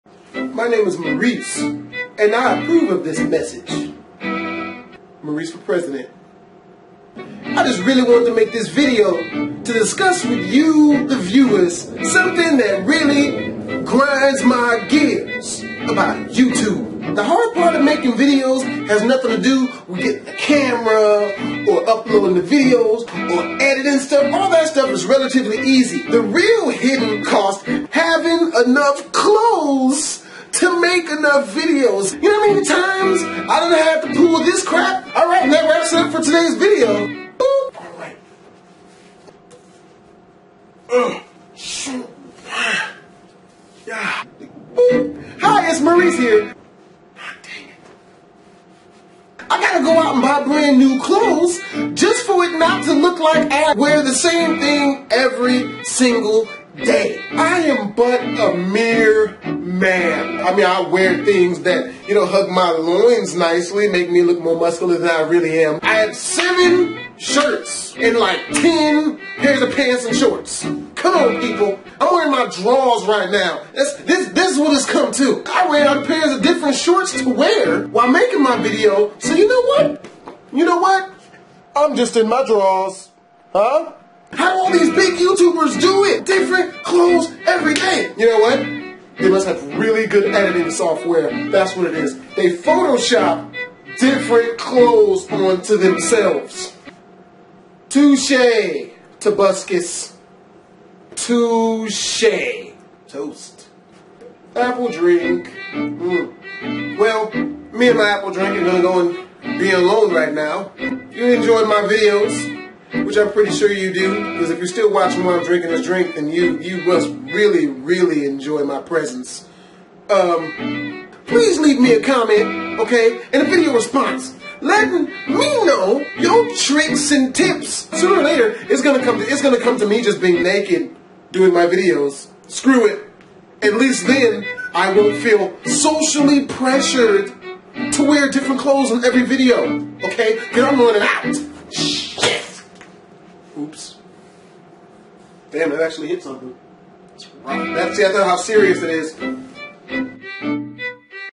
★ The Clothing Struggle(Fashion Rant) w/ Maurice Barnett ★
the clothing struggle is about not having enough clothing on youtube lol. its the clothing struggle and im just gonna be naked now.